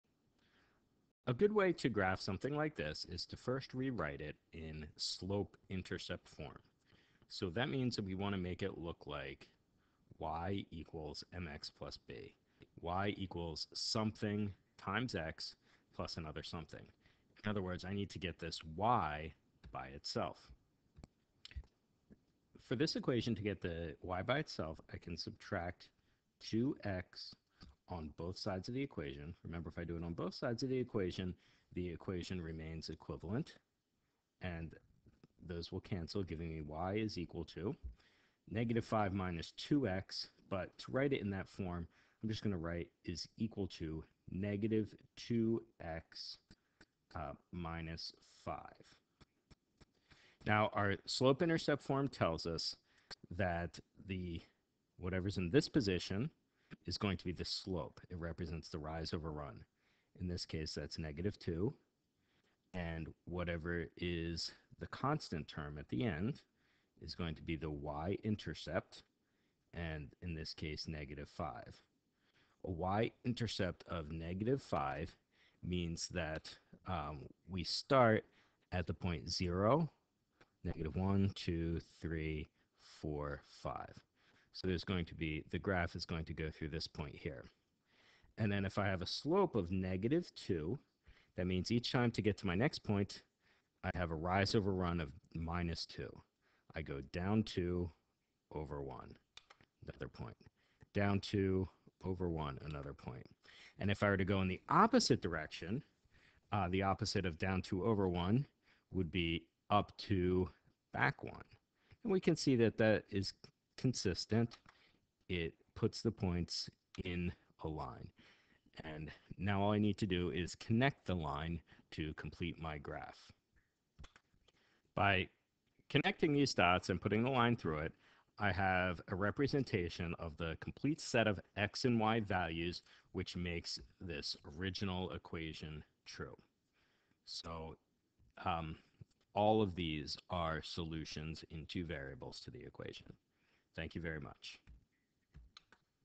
Graph 2x + y = -5
Example of how to graph an equation by first writing it in slope intercept form.